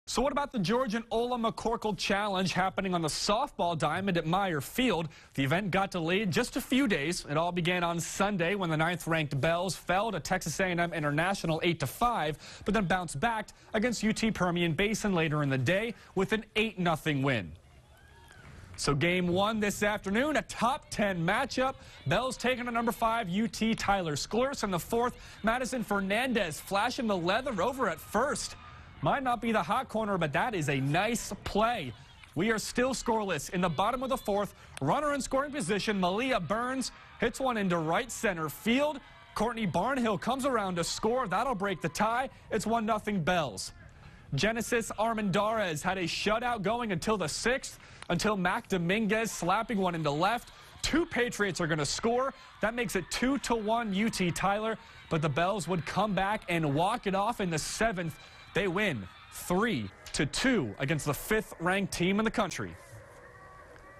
HIGHLIGHTS: No. 9 Angelo State walks it off against No. 5 UT-Tyler
The 9th ranked Belles softball team walk it off to upset No. 5 UT-Tyler at the McCorkle Challenge on Monday.